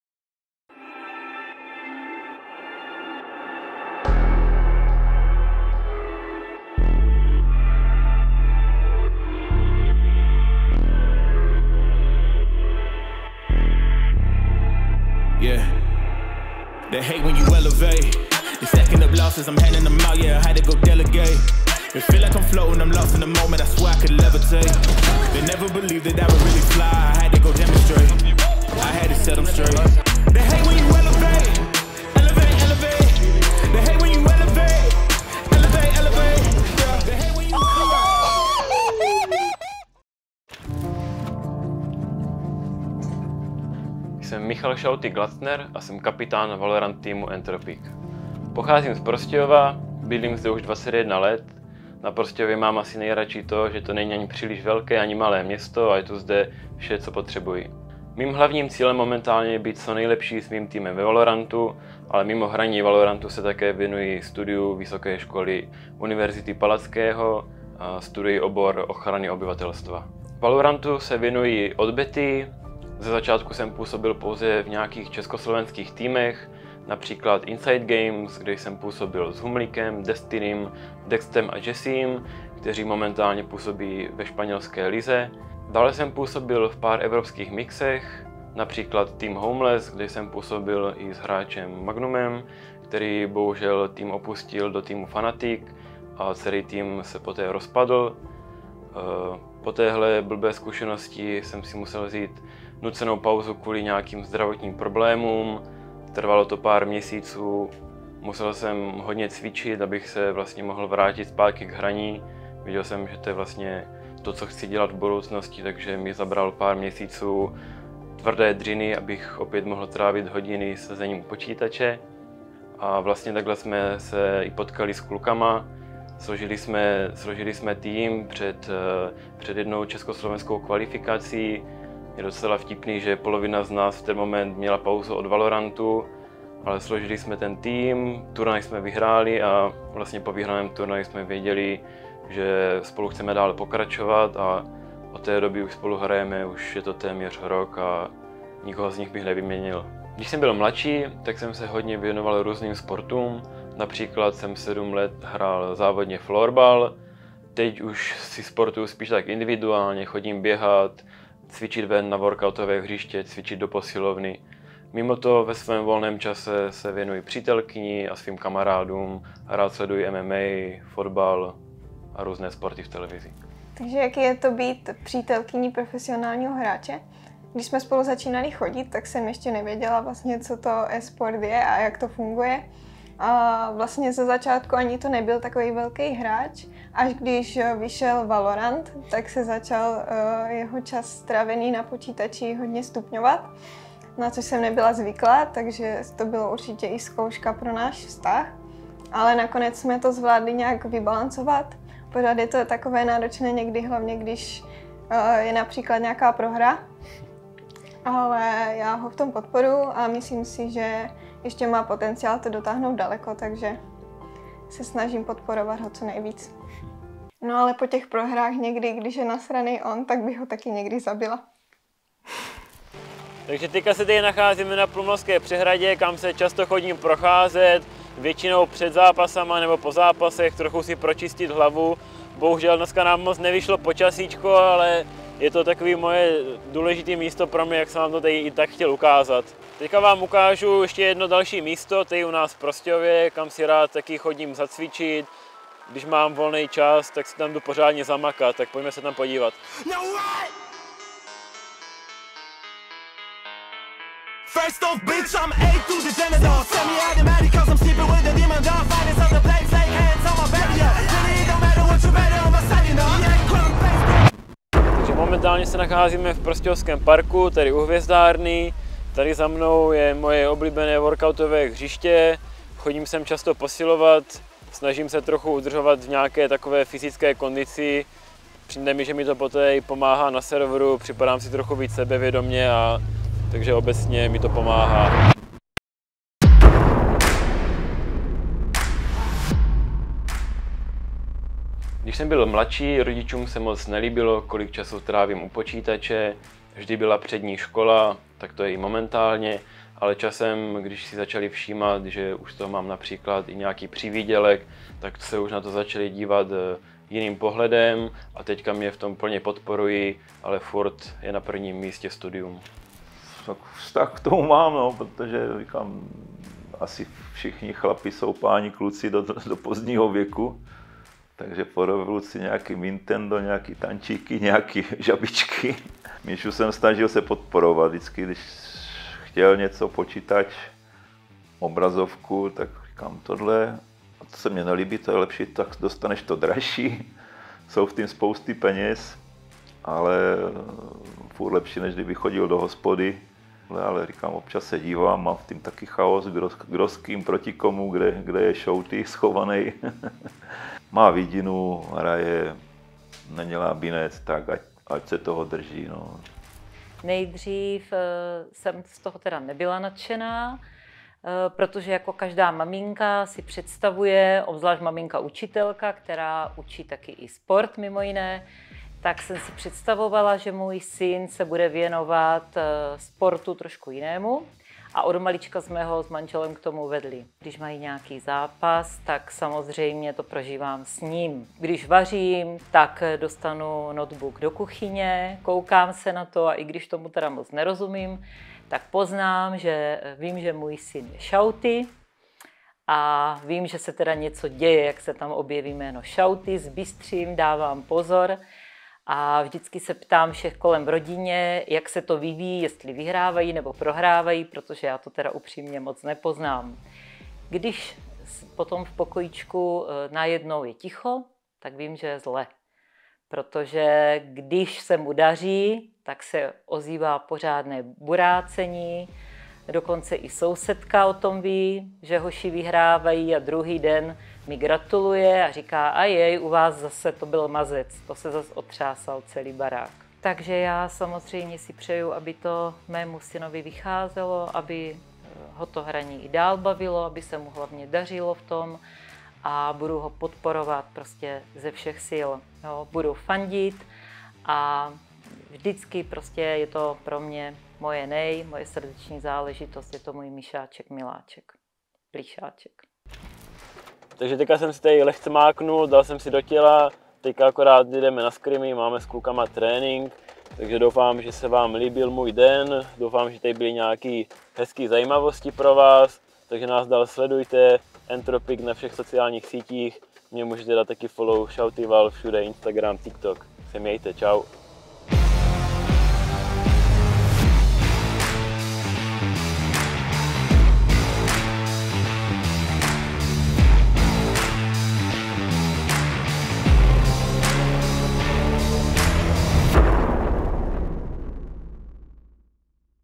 Z Florbalisty Valorant Profíkem | Meet the Player Ep.11 Shouty
Meet The Shouty. Další díl série ve které vás přivádíme k hráčům do jejich života je tu. V tomto díle o kapitánovi naší Valorant sekce Michalovi "Shoutym" Glacnerem.